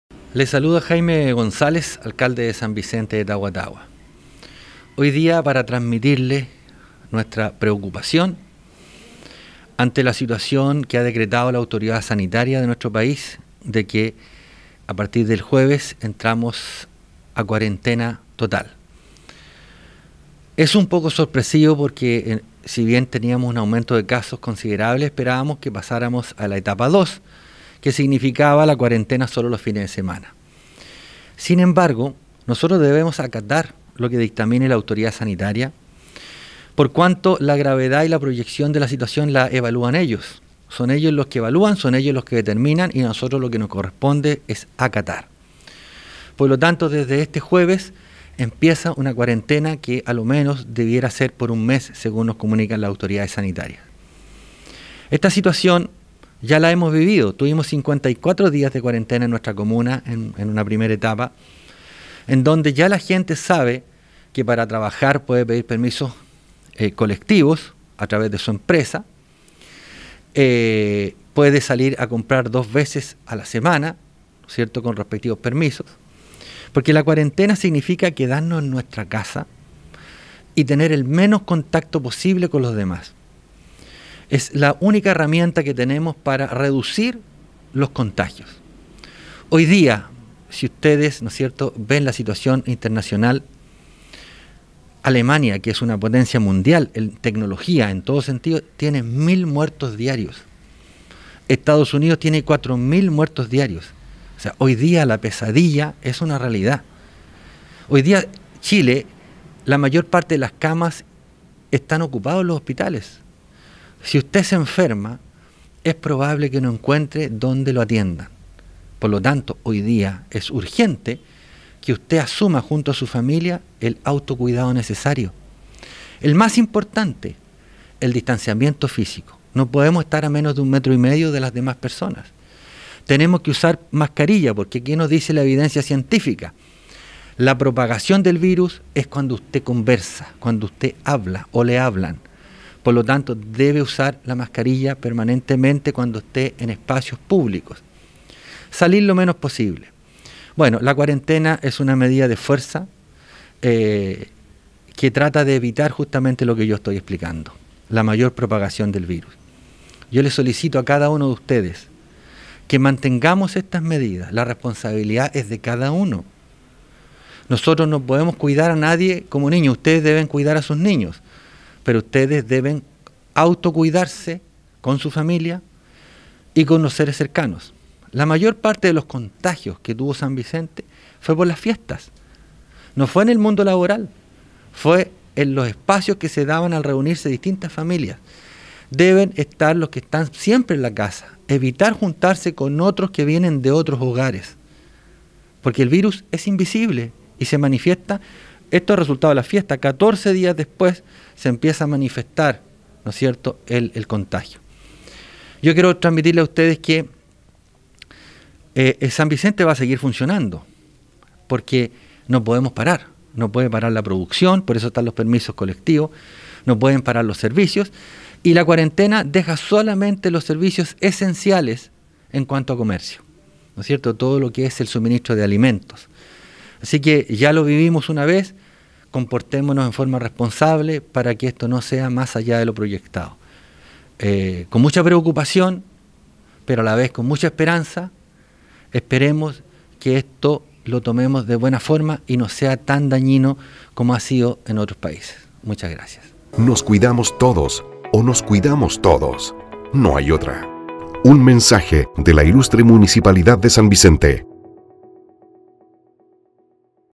ALCALDE JAIME GONZÁLEZ LLAMA A LA RESPONSABILIDAD Y AUTOCUIDADO ANTE EL RETROCESO A CUARENTENA
Preocupado, pero a la vez esperanzado se mostró el alcalde, Jaime González Ramírez, ante el anuncio de que San Vicente retrocederá a cuarentena a partir de las 05:00 horas de este jueves 14 de enero.

Según indicó la primera autoridad comunal, dicho anuncio lo tomó por sorpresa, ya que a pesar del sostenido aumento de casos esperaba un retroceso a transición, pero no directamente a la fase más restrictiva del plan “Paso a paso”. Sin embargo, y ante los hechos, hizo un fuerte llamado a respetar esta nueva etapa de cuarentena en la comuna, puesto que es la única manera de disminuir los casos de Covid-19 en San Vicente y poder avanzar lo más pronto posible a la fase 2.

Asimismo, instó a la comunidad a ser responsables ante esta situación actual de la pandemia, tomando como ejemplo lo que ocurre actualmente en otros países, en donde a pesar de su desarrollo, por estos días registran mil y hasta 4 mil muertos diarios, como es el caso de Alemania y Estados Unidos, respectivamente.

Agregó también, que la responsabilidad es personal, por lo que el autocuidado es primordial para que, efectivamente, nos cuidemos, cuidemos a nuestros seres queridos y esta nueva etapa de cuarentena sea lo más acotada posible: “La cuarentena ya la vivimos una vez, comportémonos en forma responsable para que esto no sea más allá de lo proyectado”, finalizó.